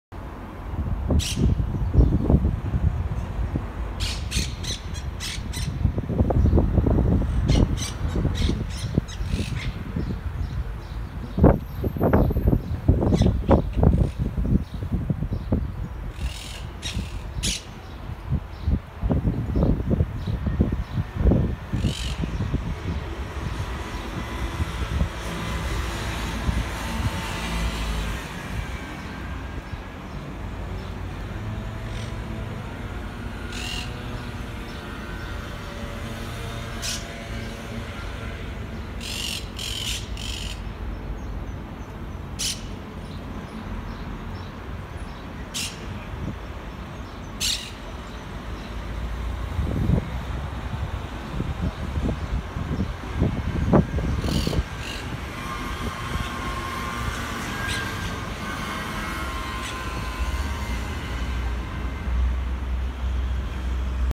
Monk Parakeet at Malaga Spain
Monk Parakeet in Malaga outside the apartment I’m staying.